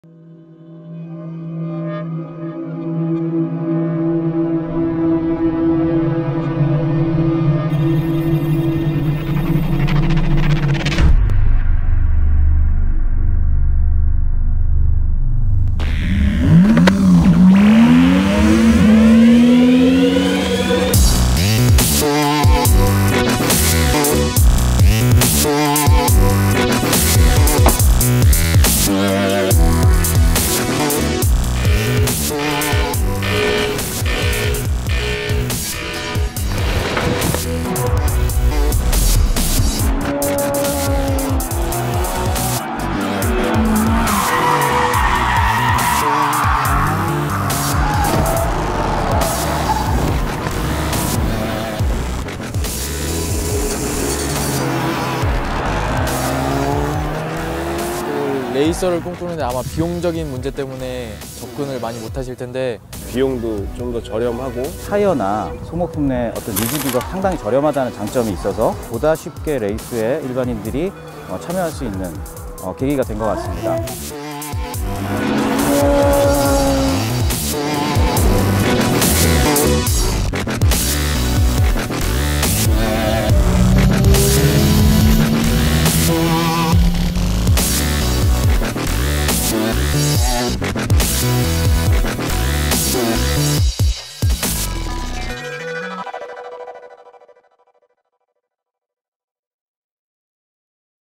2018 Korea Speed Festival ‘모닝 챌린지 레이스’
누구나 부담없이 출전 가능한 국내 최고 경차 레이스,
모터스포츠 입문을 꿈꾸는 드라이버들의 꿈의 무대가 현실로!

대한민국 대표 경차 모닝이 펼치는 서킷 위의 진검 승부
국내 최대의 경차 레이싱의 뜨거운 열기를 확인하세요!